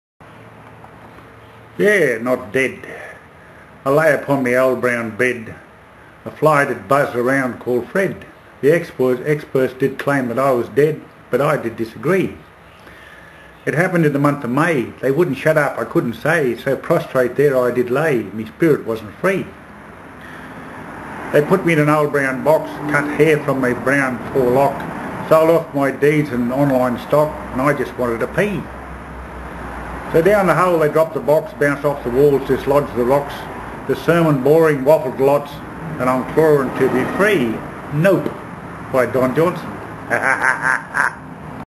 Guess i'm not dead
NOT .DEAD..
I lay upon me old brown bed.
a fly did buzz around called Fred.
The experts did claim that I was dead.
But I did disagree...

It happened in the month of may...
They wouldn't shut up, I couldn't say...
So prostrate there I did lay...
Me spirit wasn't free.

She put me in an old brown box...
Cut hair from my brown fore lock...
Sold all my deeds and online stock...
But I just wanted to pee.

So down the hole they dropped the box ...
Bounced off the walls dislodged the rocks.
The sermon boring, waffled lots...
I'm clawing to be free. nope.

Don Johnson09.
Baggy ass...
Did u ever stop to wonder about the baggy ass old bean ...
It was in the sixties when we had the skin tight class of jeans.
When it was cool the skin tight jeans and body shirts so fine ...
When you could see so shapely the women were sublime...

The army had the baggy ass the bloody Bombay bloomers..
The soldiers hated baggy ass they looked like crap on tuna..
In the seventies   fashion brought out baggy ass no one would buy it then...
It wasn't cool no hippy fool would have it wear it ..it offend...

So brainwash the public its cool, just wear this floppy sack..
With your baggy knee length shorts a flopping forth and back...
Shapeless bland and worthless ,rag bag ,bloody crapppp..
No class or style about it, a hanging on your rack?...
Don Johnson .